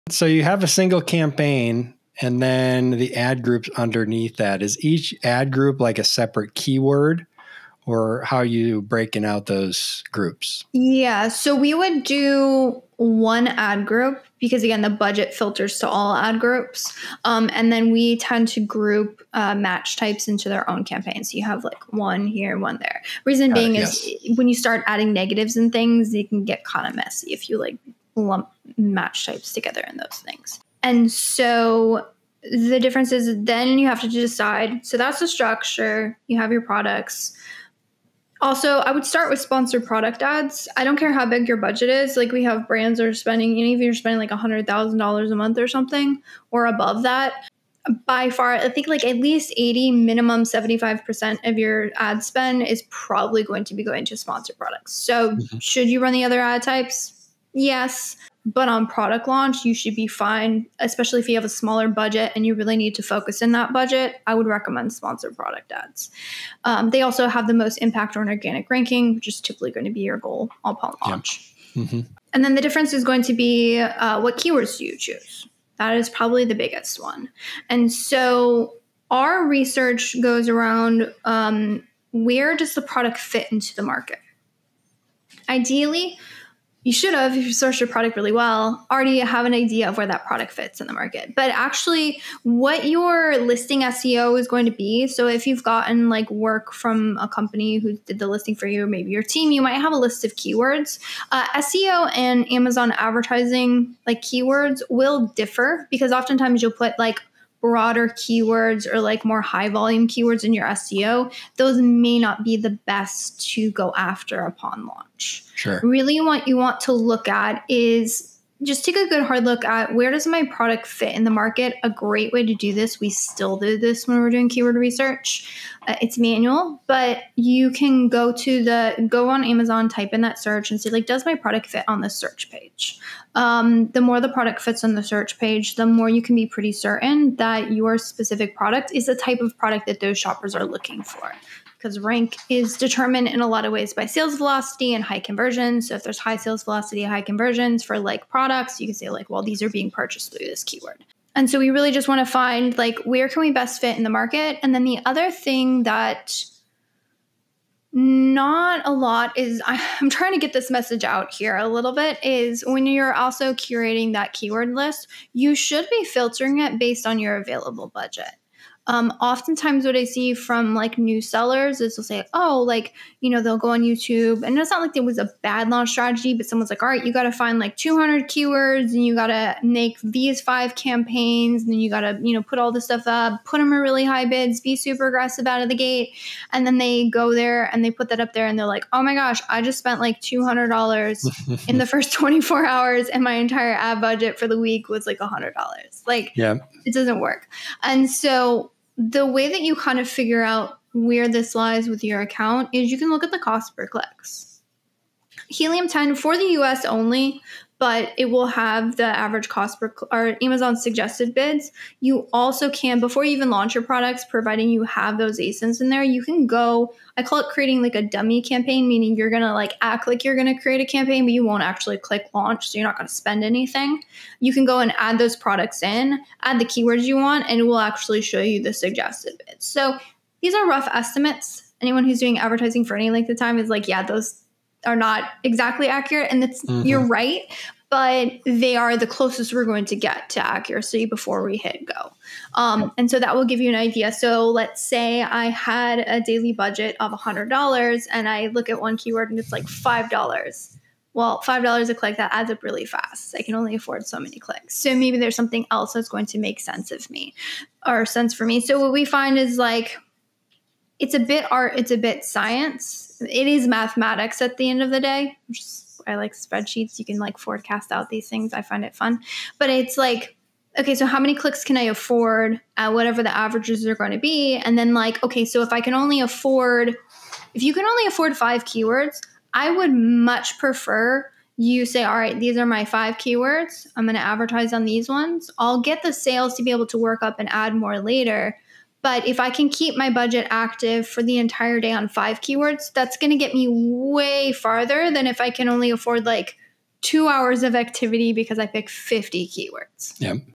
Optimizing Amazon Ad Campaigns with Budget-Friendly Keyword Selection
When structuring Amazon ad campaigns, group match types into separate campaigns for easier management and effectiveness. For most sellers, especially those launching a product, Sponsored Product Ads are the top priority due to their impact on organic rankings. Begin by choosing a few high-relevance keywords based on where your product best fits in the market. Avoid overwhelming budgets by targeting fewer keywords with consistent activity throughout the day, rather than scattering budget thinly across many keywords. Tools like Helium 10, suggested bid estimates, and even "dummy campaigns" can help plan your strategy within budget for maximum exposure.